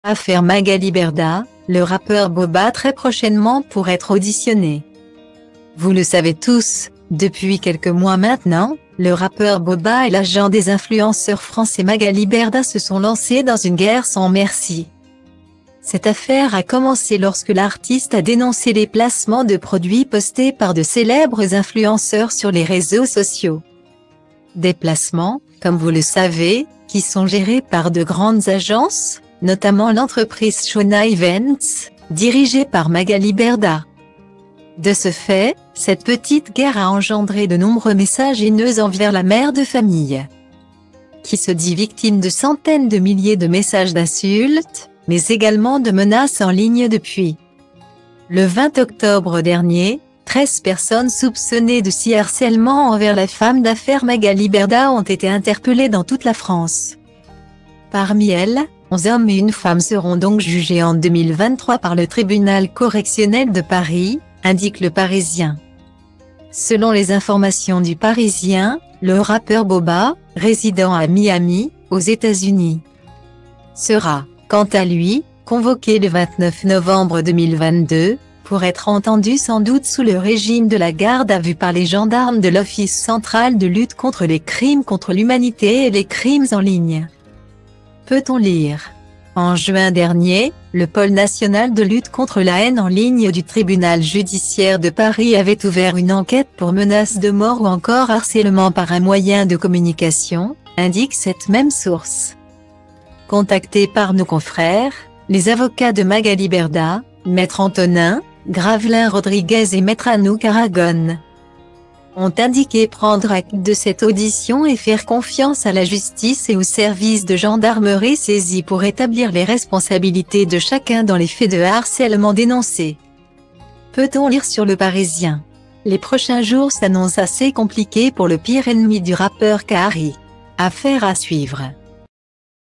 Affaire Magali Berdah : le rappeur Booba très prochainement pour être auditionné
Vous le savez tous, depuis quelques mois maintenant, le rappeur Booba et l'agent des influenceurs français Magali Berdah se sont lancés dans une guerre sans merci.Cette affaire a commencé lorsque l'artiste a dénoncé les placements de produits postés par de célèbres influenceurs sur les réseaux sociaux.Des placements, comme vous le savez, qui sont gérés par de grandes agences, notamment l'entreprise Shauna Events, dirigée par Magali Berdah.